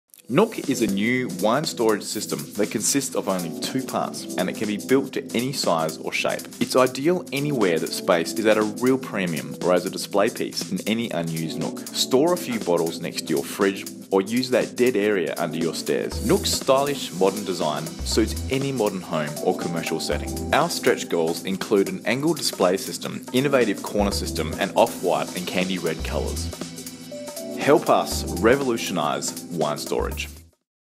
Introducing Nook, Modular Wine Storage For any Nook: Indiegogo brief Overview
We are launching in early November on Indiegogo, here is our brief overview product video, more information will come as we get closer!

Nook is a clever, simple wine storage system made up of just two components that you assemble to fit anywhere! It has a stylish, modern design and it's affordable! Sizes available to suit 6 bottles or 60,000!